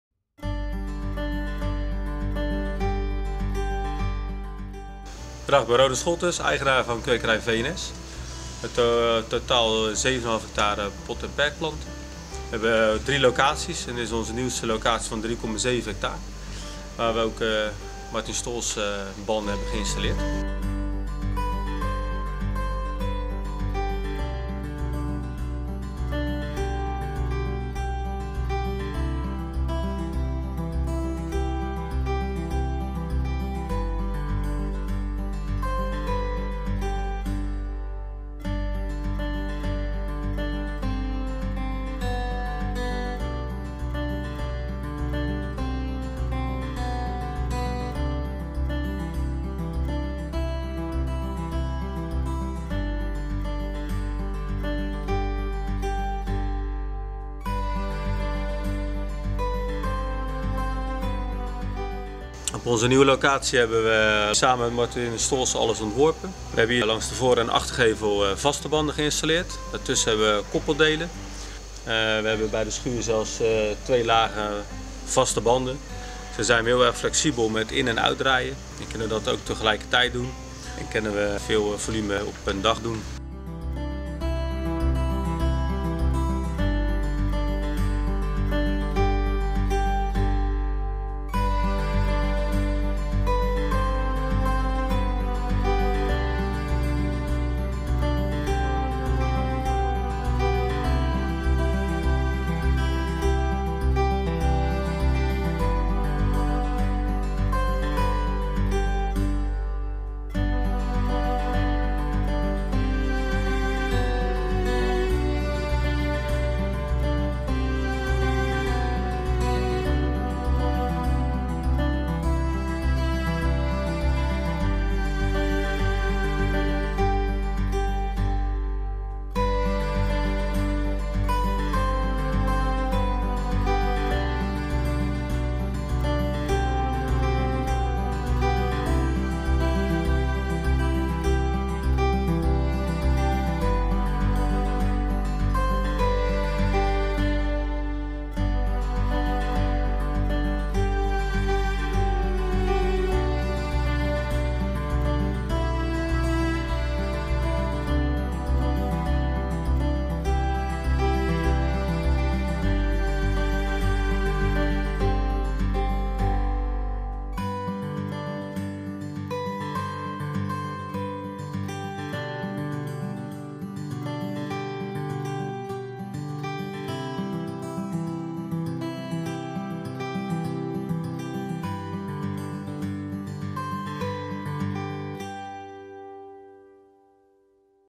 Pottingmachine & conveyor system - Topfmaschine & Förderbander -Oppotmachine & transportbanden
In early 2020, Ronald Scholtes of Scholtes Roses joined Kwekerij V&S. This doubled the total area of Kwekerij V&S from 3.8 ha to 7.5 ha. The Scholtes Roses rose nursery has been converted into a pot and bedding plant business. Martin Stolze has been able to contribute to the set-up of this location.

Concurrent potting and delivery
Ronald Scholtes asked Martin Stolze BV for a flexible system for potting, staking and delivering. We came up with a solution together, in which these activities can be carried out concurrently. We added a double-layer main belt to our Stolze 3030 potting machine which enables multiple routes. The Stolze 3030 potting machine is also prepared for the installation of a fully automatic planting machine in the future. Ronald Scholtes: ‘It’s a high-quality and flexible system.’

More than a kilometre
We installed a combination of more than a kilometre of fixed conveyor belts and EasyMax motor and coupling sections. This is all controlled from the central control unit, with a clear touch screen for setting up the routes. The system can be started and stopped via a radio remote control.